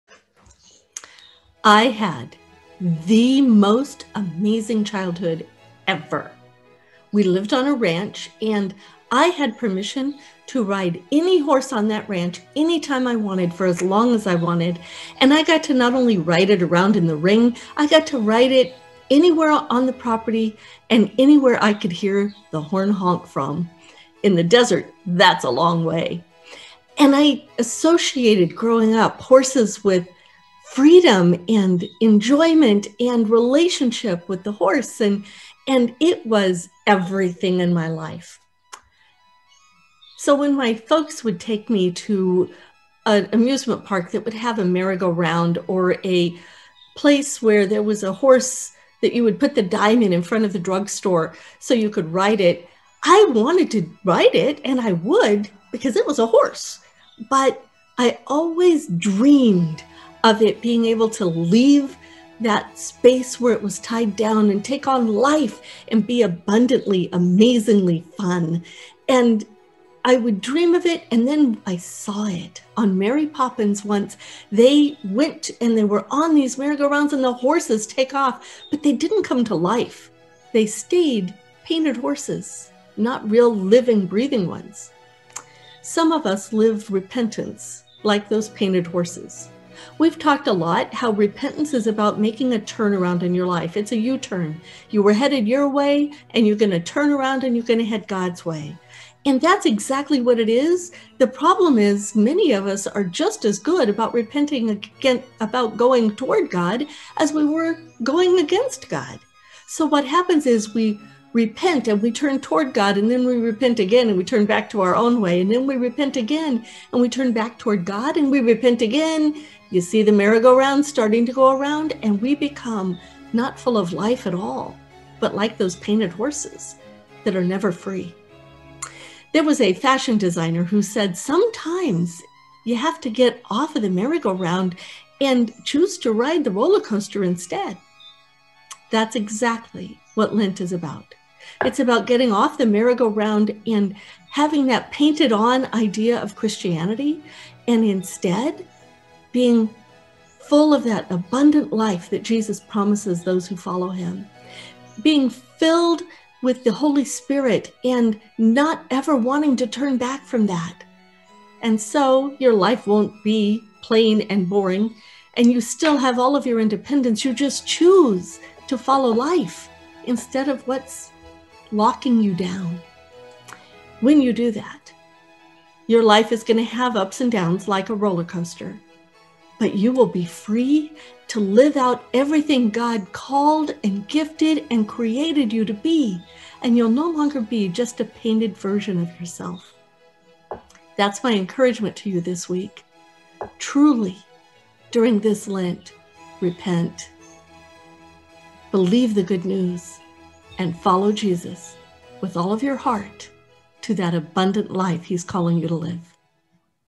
Get Off the Merry-Go-Round | Devotional
Do you ever feel like every time you try to get right with God, it seems like you're going in circles? Does it feel like God is standing by the merry-go-round of your life and you see Him on each pass, but can't get off and connect?

If this sounds familiar, then this devotional is for you!